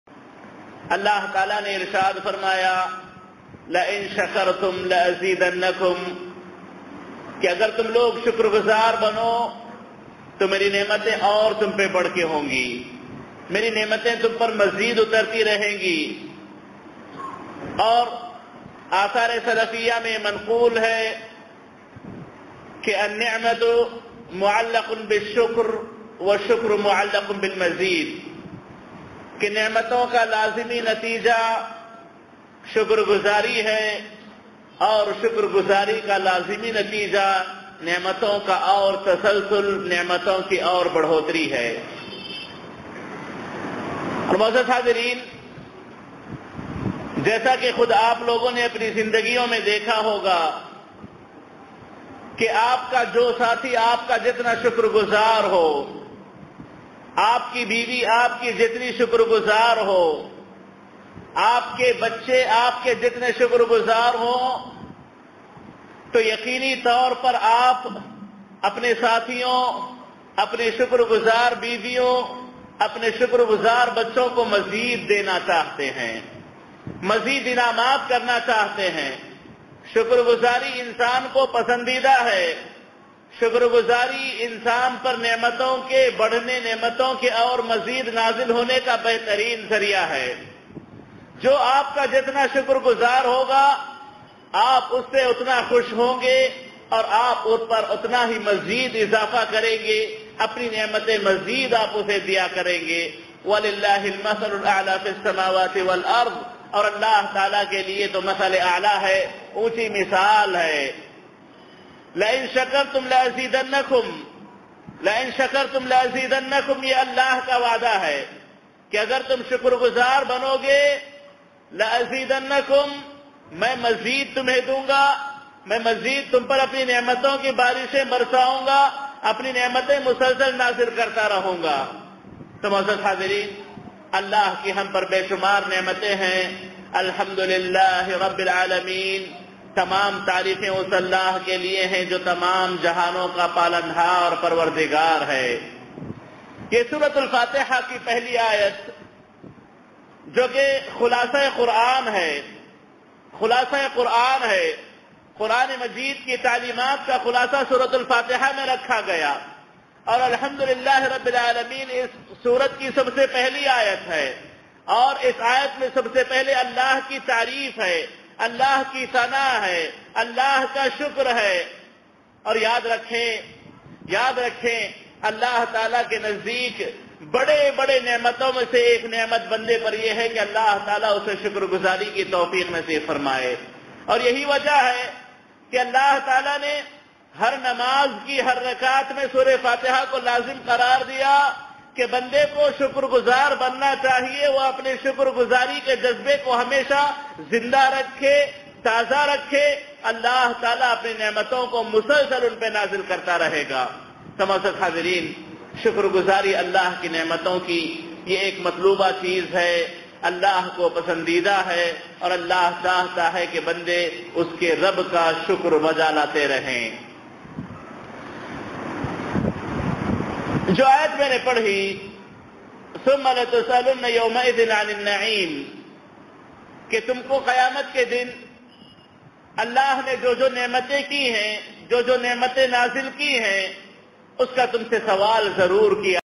Nematon Ko Badhana Chahte Ho To | Shaykh Tariq Saudagar Madani
This clip is taken from the lecture:
Paani - Ek Azeem Nemat

٠٠٠٠٠٠٠٠٠٠٠٠٠٠٠٠٠٠٠٠٠٠٠٠٠٠٠٠٠٠٠٠٠٠٠٠٠٠٠٠